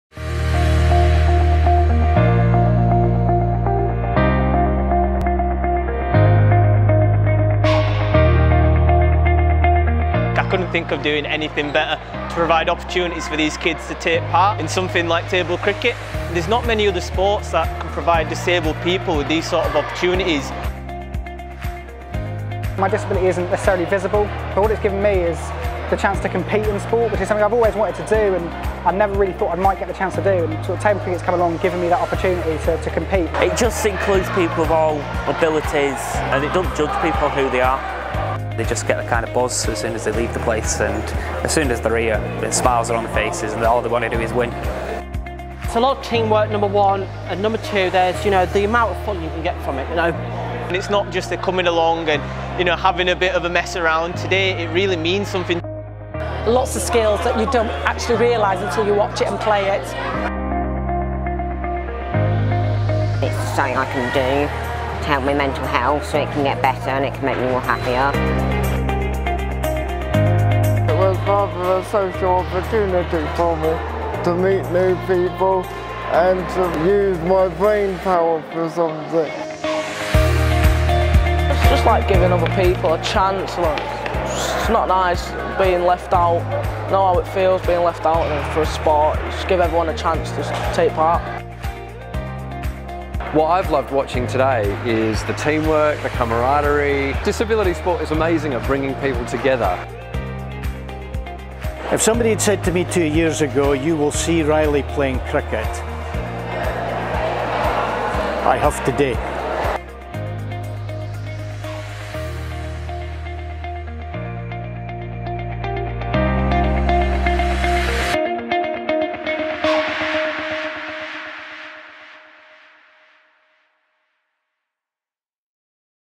Table Cricket Sizzle Reel
Take a look at how Table Cricket has been providing disabled young people an opportunity to play regular competitive cricket